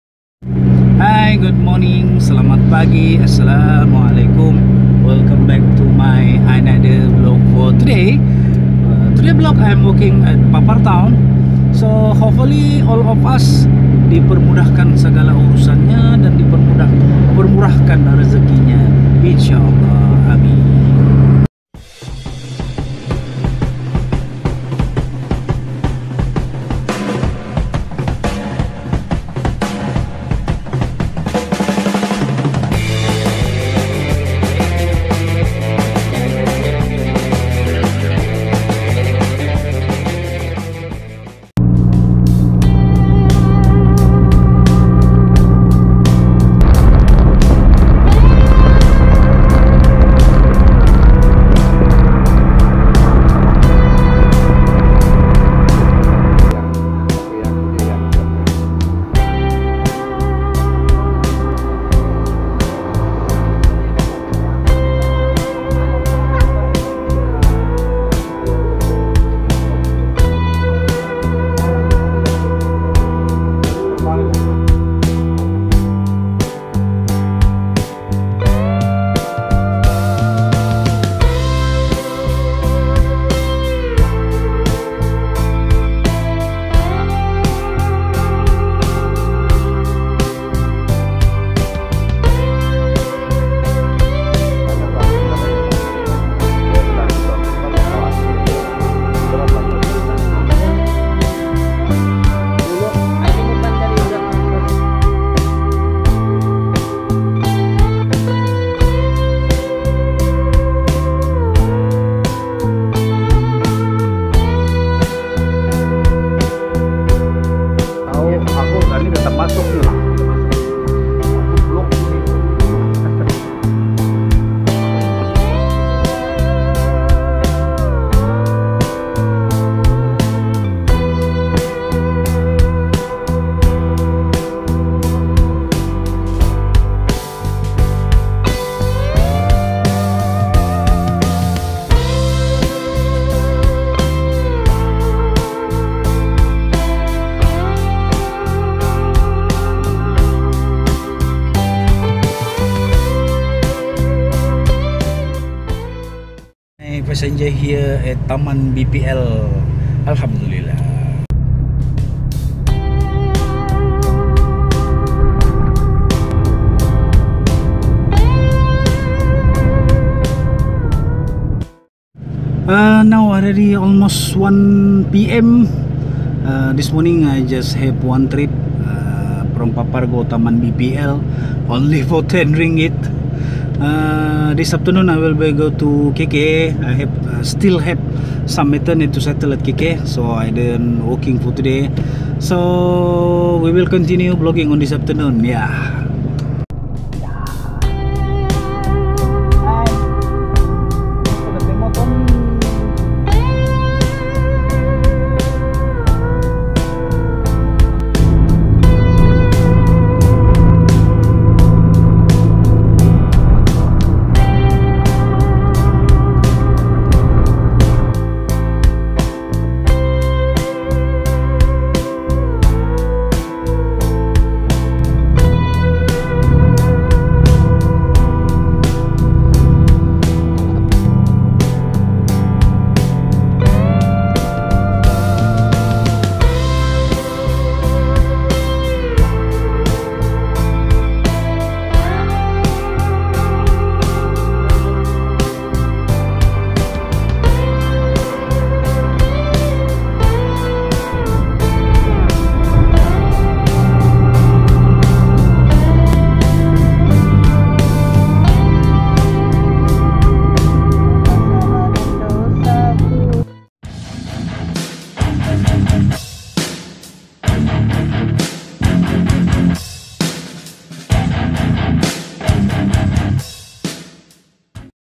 Hari-Hari Biasa Alhamdulillah
Today nothing more interest but yer alhamdulillah. Hanya hari-hari biasa. Mensyukuri sebuah kehidupan itu adalah sesuatu yang baik.